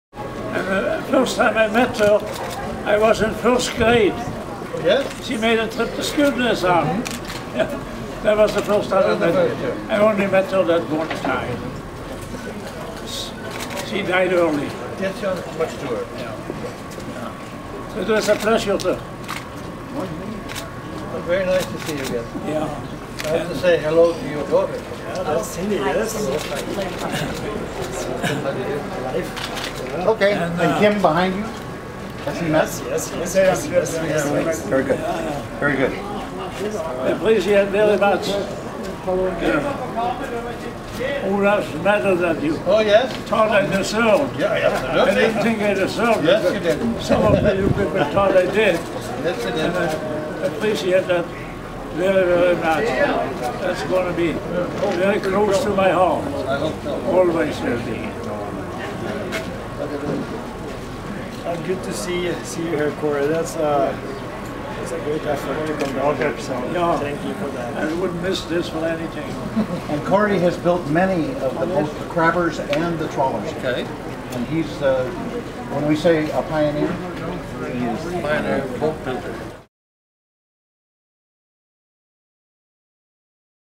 Video Hjertelig gjensyn med Kongen HD
King Harald V of Norway  spends a few moments with Kaare Nes, owner of the MARCO 108’ Alaskan King Crabber F/V ROYAL VIKING & founder of Trident Seafoods.